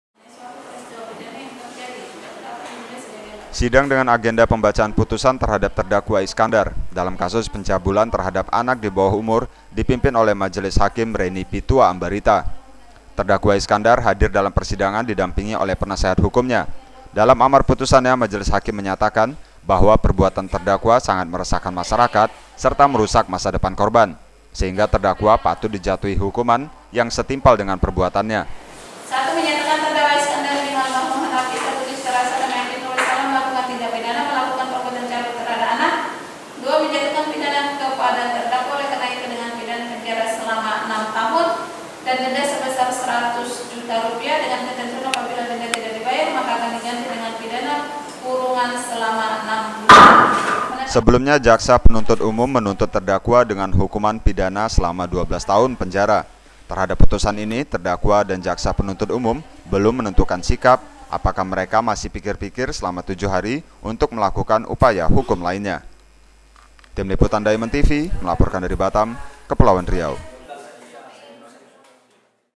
Cabuli Anak di Bawah Umur, Iskandar di Hukum 7 Tahun Penjara
DiamondOneNews, Batam - Sidang dengan agenda pembacaan putusan terhadap terdakwa Iskandar, dalam kasus pencabulan terhadap anak di bawa umur di pimpin oleh majelis Reni Pitua Ambarita dan dua anggotanya Taufik Nainggolan serta Egy Novita serta jaksa penuntut umum Nani Herawati.
Sementara itu terdakwa Iskandar hadir dalam persidangan di dampingi oleh penasehat hukumnya.
Dalam amar putusannya, majelis hakim menyatakan bahwa perbuatan terdakwa sangat meresahkan masyarakat serta merusak masa depan korban, sehingga terdakwa patut dijatuhi hukuman yang setimpal dengan perbuatannya.
Menurut ketua majelis hakim unsur pasal 82 ayat (1) undang-undang ri nomor 35 tahun 2014 tentang perubahan atas UU RI nomor 23 tahun 2002 tentang perlindungan anak, yang didakwakan jaksa penuntut umum sudah terpenuhi sehingga tak ada alasan untuk membebaskan terdakwa dari dakwaan tersebut.
Hukuman tersebut lebih ringan enam tahun dari tuntutan JPU yang sebelumnya menuntut agar terdakwa di hukum dengan pidana penjara selama 12 tahun.
Terhadap putusan ini terdakwa dan jaksa penuntut umum belum menentukan sikap, mereka masih pikir-pikir selama 7 hari untuk melakukan upaya hukum lainnya.
(red)